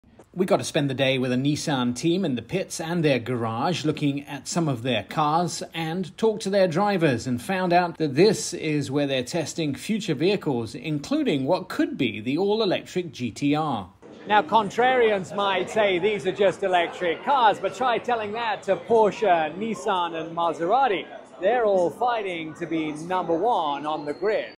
Inside the Race: Electric Dreams and Fierce Rivalry
Join us for an exhilarating journey into the fast-paced world of racing! Experience thrilling on-track action and dynamic camera angles as we delve into the competitive spirit among leading car manufacturers such as Nissan, Porsche, and Maserati. Discover electric innovations set to revolutionize the racing scene!